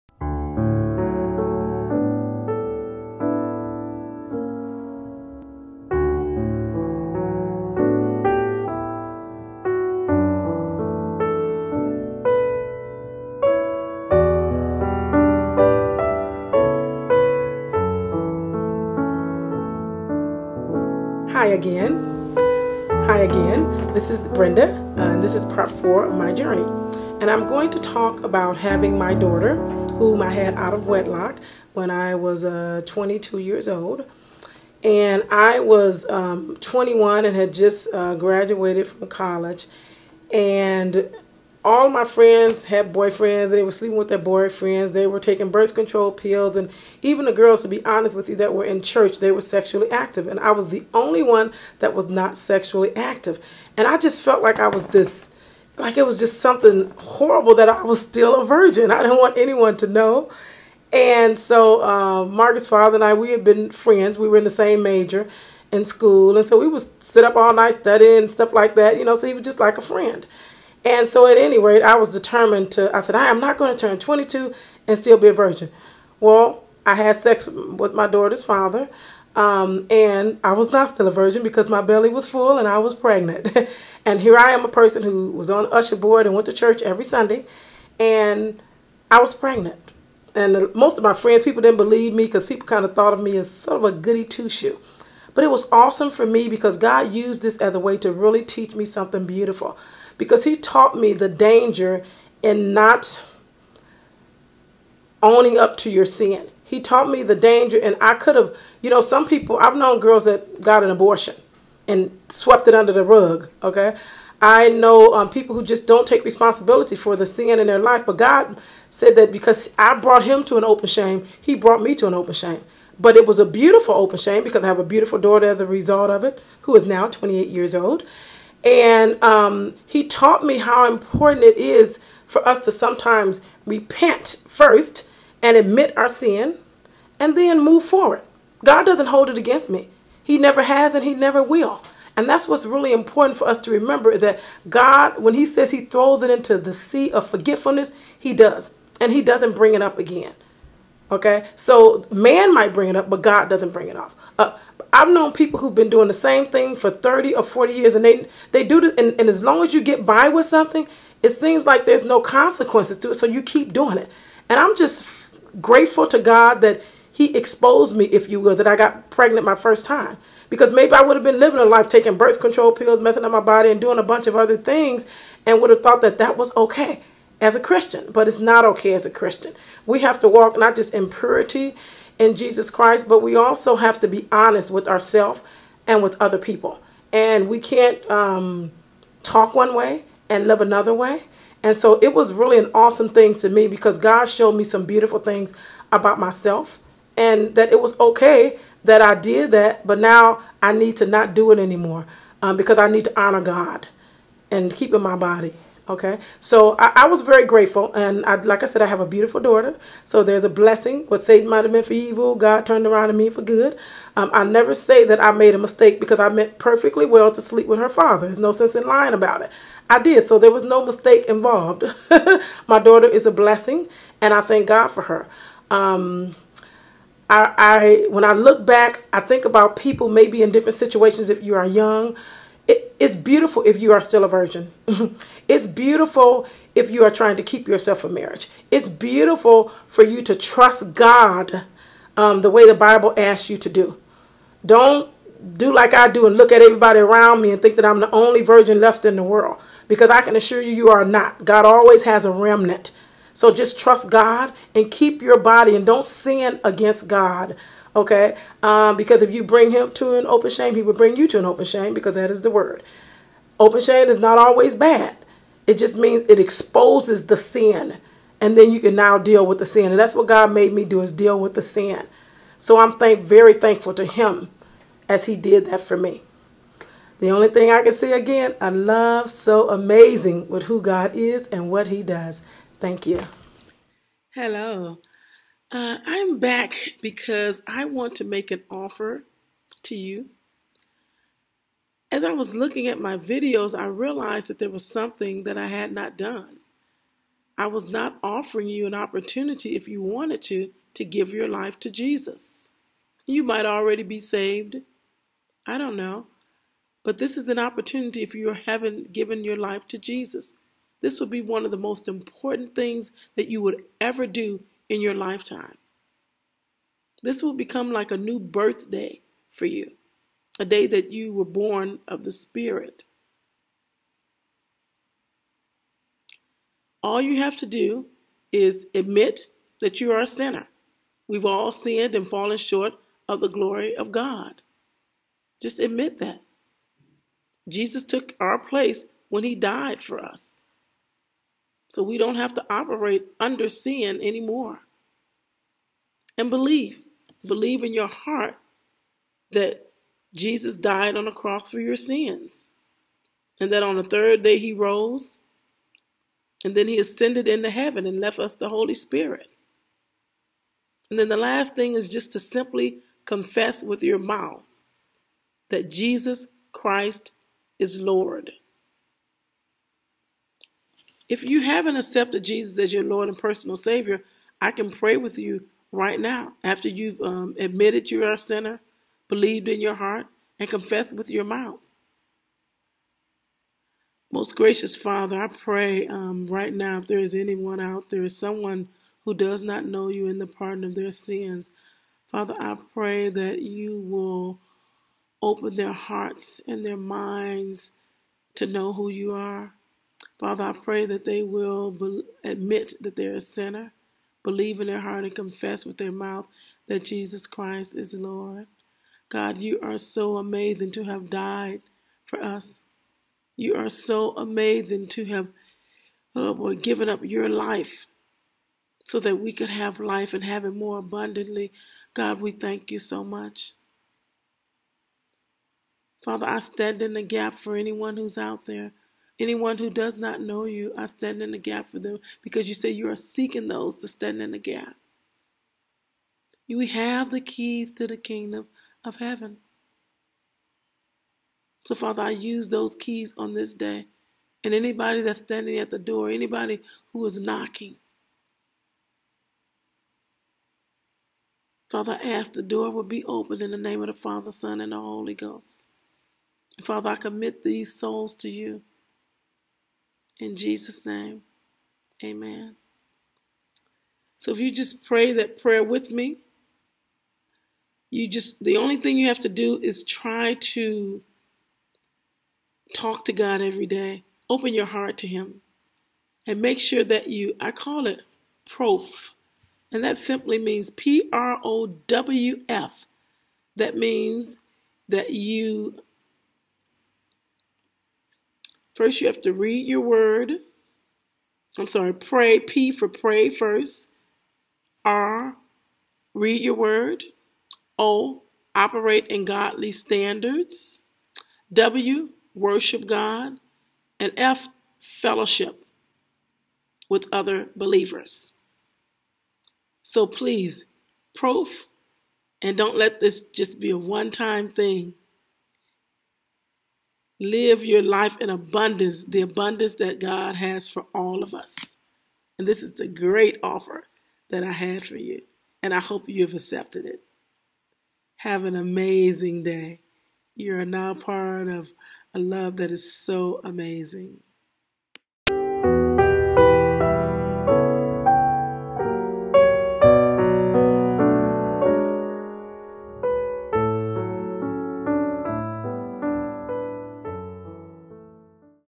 My Daughter--My Journey-Testimony 4--Dr. Brenda Joyce Sanders
This is number 4 in a 10-part series of testimonies that God laid on my heart to share. These were taped 5 years ago.